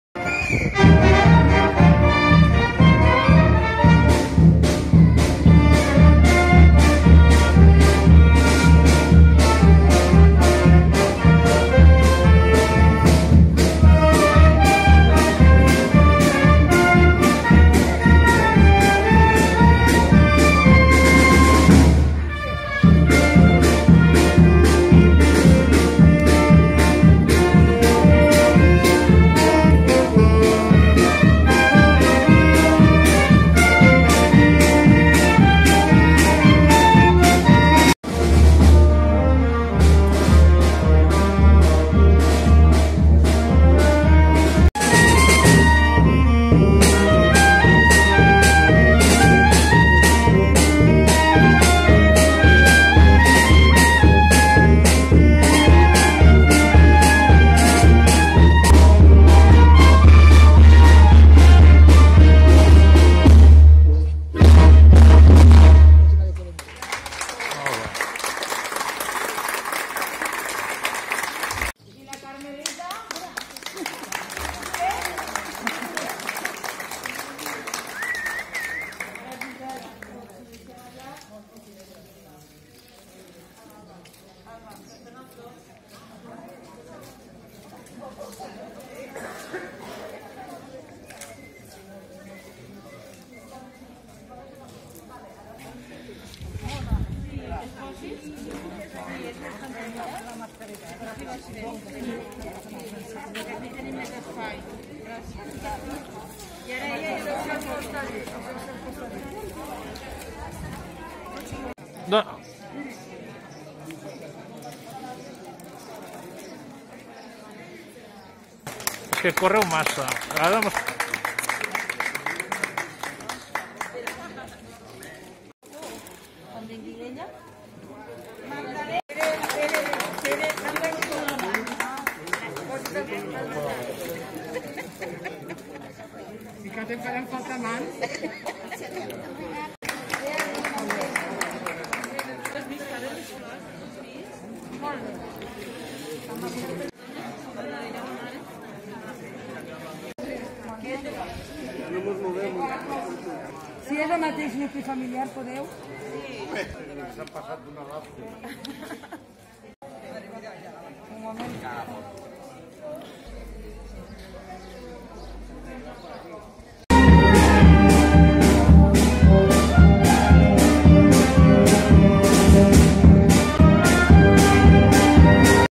HOMENATGE A LA VELLESA PLAÇA DEL MERCAT
Homenatge a la Vellesa. Dins dels actes de la Festa Major de Sant Jaume del 2020, un dels importants, és l'homenatge que es fa als veïns dels 80 anys cap amunt. Aquest any l'Homenatge també s'ha vist afectat per la COVID 19, i s'ha fet, concentrant als homenatjats en diferents carrers i places del poble, de manera que en cap lloc hi hagués una alta concentració de persones.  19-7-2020